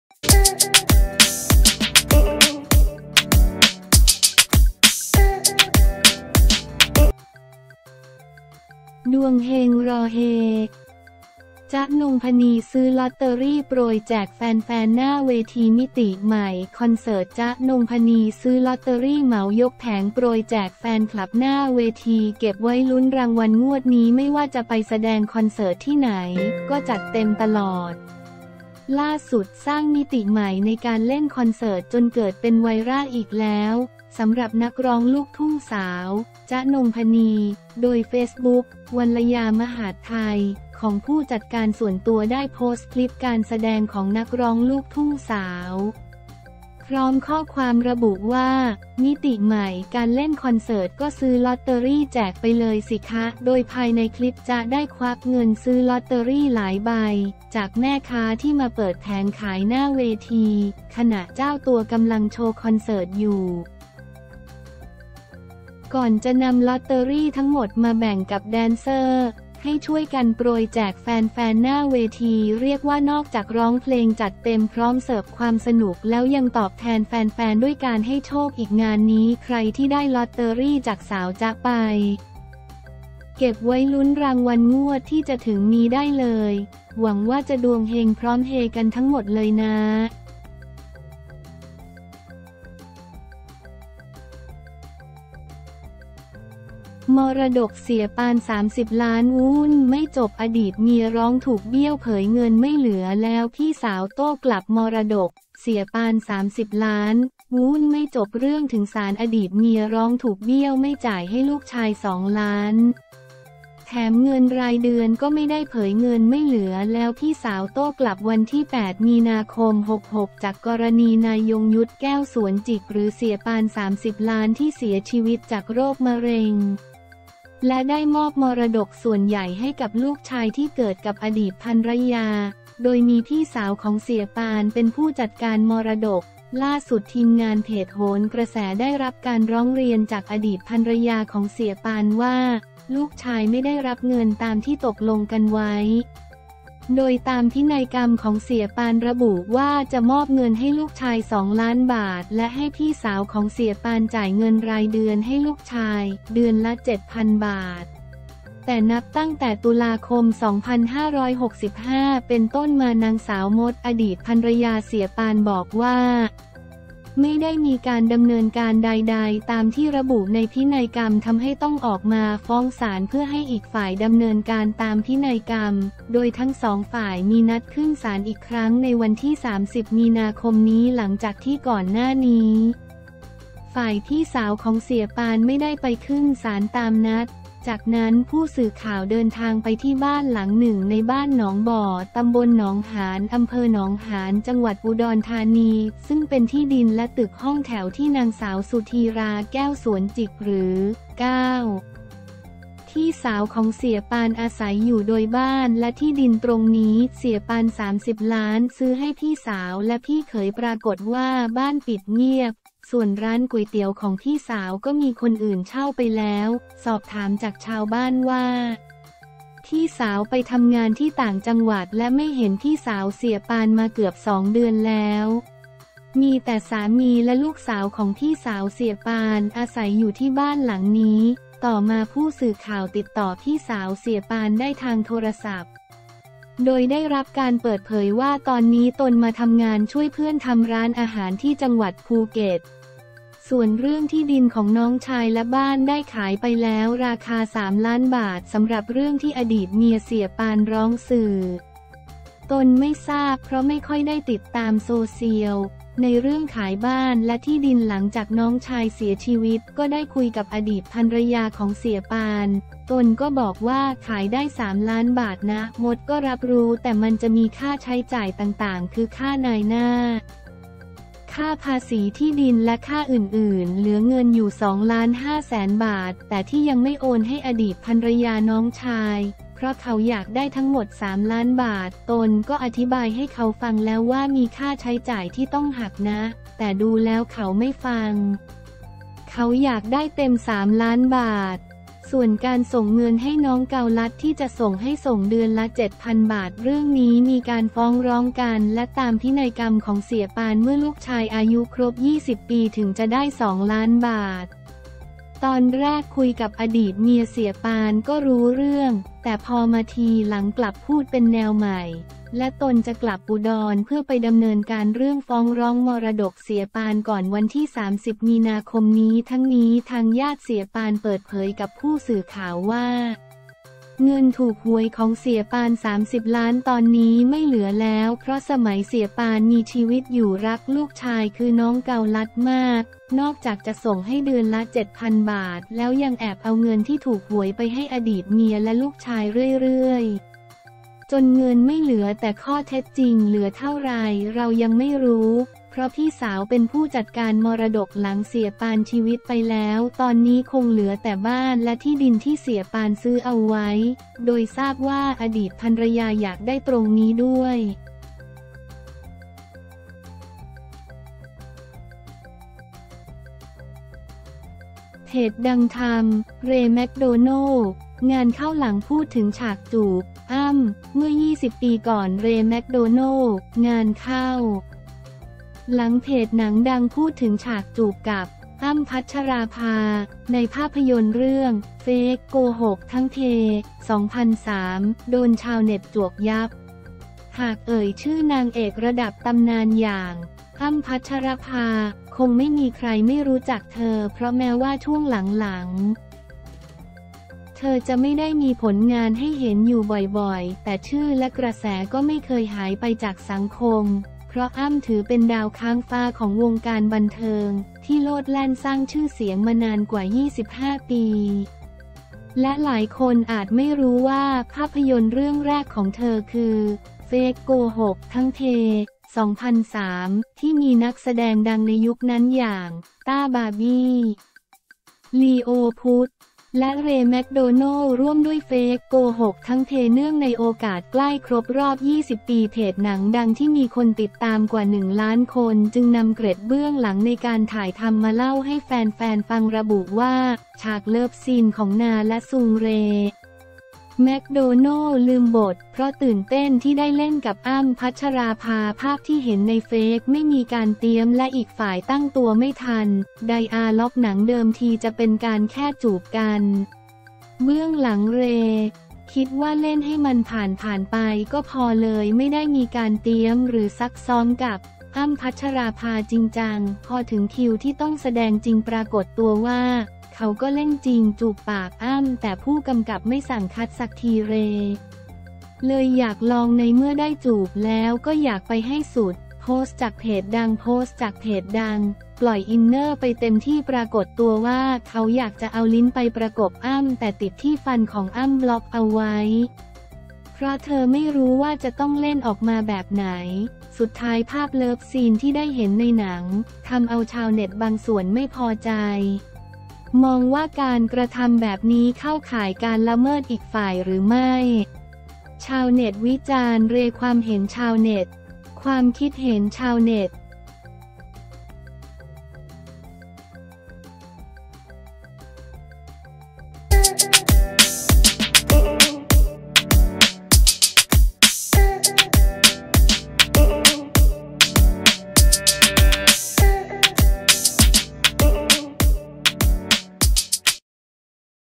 ดวงเฮงรอเฮ! จ๊ะ นงผณี ซื้อลอตเตอรี่โปรยแจกแฟน ๆ หน้าเวที
มิติใหม่คอนเสิร์ต จ๊ะ นงผณี ซื้อลอตเตอรี่เหมายกแผง โปรยแจกแฟนคลับหน้าเวที เก็บไว้ลุ้นรางวัลงวดนี้
ไม่ว่าจะไปแสดงคอนเสิร์ตที่ไหน ก็จัดเต็มตลอด ล่าสุดสร้างมิติใหม่ในการเล่นคอนเสิร์ตจนเกิดเป็นไวรัลอีกแล้ว สำหรับ นักร้องลูกทุ่งสาว “จ๊ะ นงผณี“ โดยเฟซบุ๊ก “วัลยา มหาดไทย” ของผู้จัดการส่วนตัว ได้โพสต์คลิปการแสดงของนักร้องลูกทุ่งสาว พร้อมข้อความระบุว่า “มิติใหม่การเล่นคอนเสิร์ต ก็ซื้อลอตเตอรี่แจกไปเลยสิคะ
โดยภายในคลิปจ๊ะได้ควักเงินซื้อลอตเตอรี่หลายใบ จากแม่ค้าที่มาเปิดแผงขายหน้าเวที ขณะเจ้าตัวกำลังโชว์คอนเสิร์ตอยู่ ก่อนจะนำลอตเตอรี่ทั้งหมดมาแบ่งกับแดนซ์เซอร์ ให้ช่วยกันโปรยแจกแฟน ๆ หน้าเวที

เรียกว่า นอกจากร้องเพลงจัดเต็ม พร้อมเสิร์ฟความสนุกแล้ว ยังตอบแทนแฟนๆ ด้วยการให้โชคอีก งานนี้ใครที่ได้ลอตเตอรี่จากสาวจ๊ะไป เก็บไว้ลุ้นรางวัลงวดที่จะถึงนี้ได้เลย หวังว่าจะดวงเฮงพร้อมเฮกันทั้งหมดเลยน้า